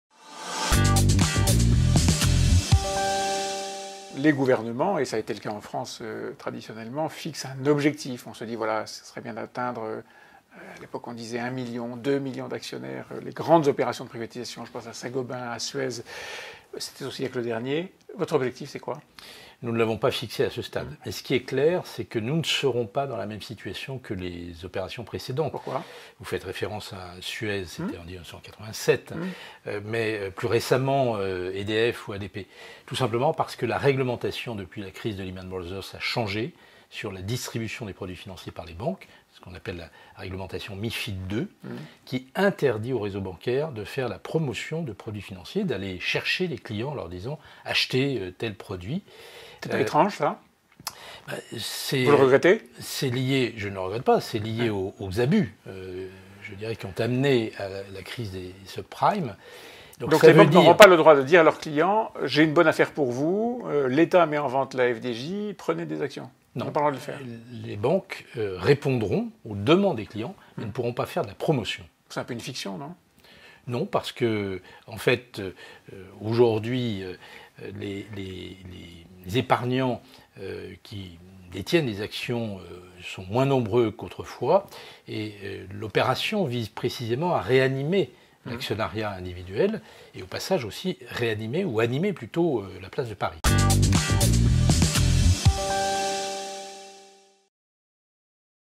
Privatisation de la FDJ: «L’opération vise aussi à réanimer l’actionnariat individuel»
Concernant la privatisation bloquée d’ADP, Martin Vial note : «Il y a depuis 2017 l’idée que l’Etat doit se concentrer en tant qu’actionnaire sur ce qui relève de la souveraineté nationale et quelques entreprises malheureusement qu’il faut secourir, comme PSA. Les autres participations, nous avons vocation à nous en dégager et à investir dans l’innovation».